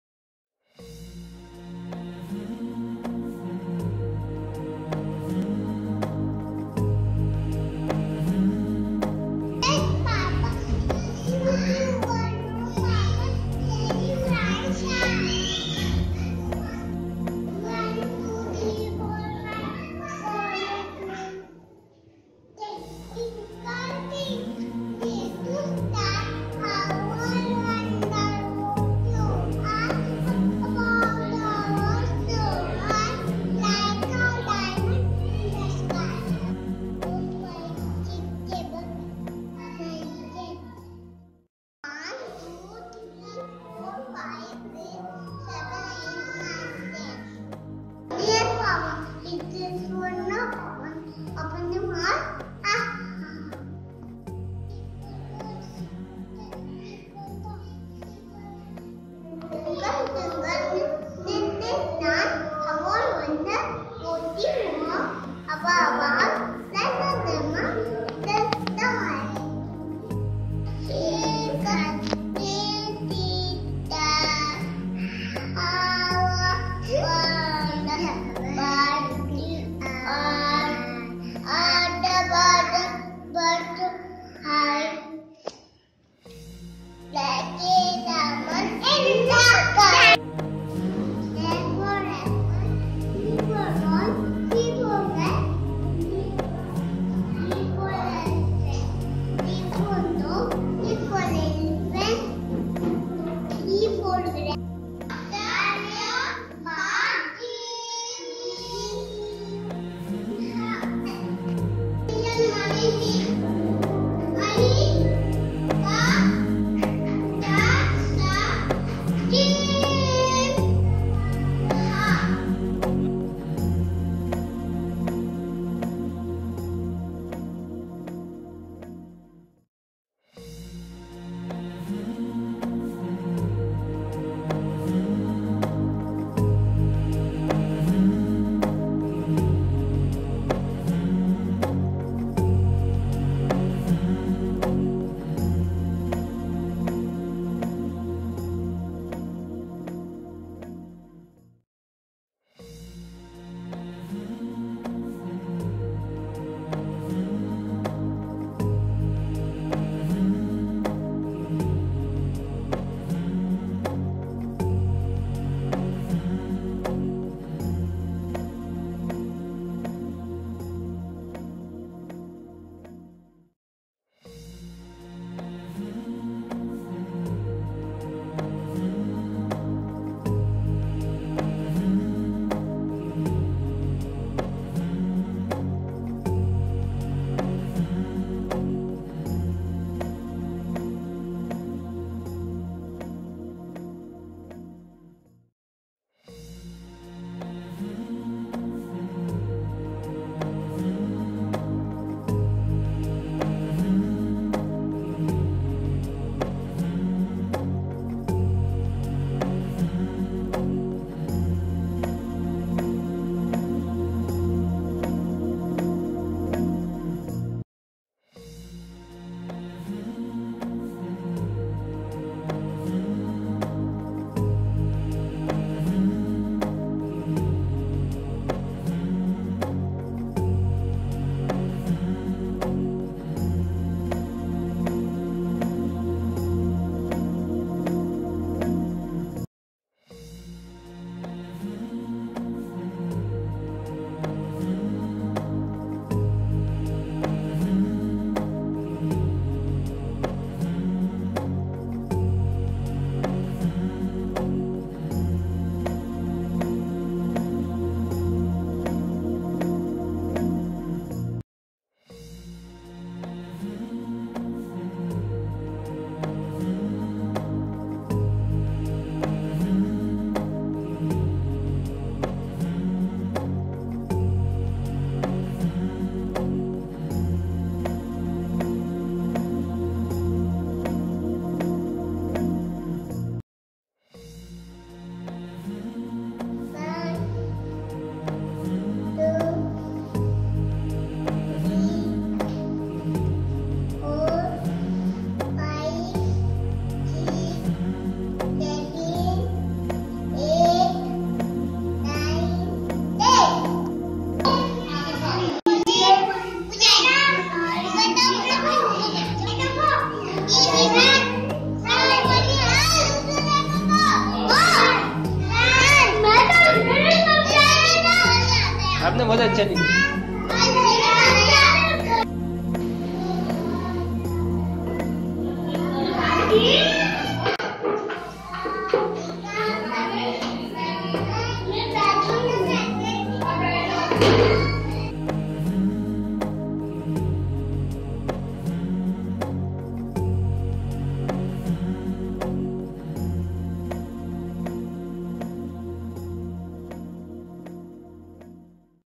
PLAY SCHOOL STUDENTS PERFORMANCE AT BIRAQ
Biraq Play School, student's performance, presentation of poem, counting, drawing, activities.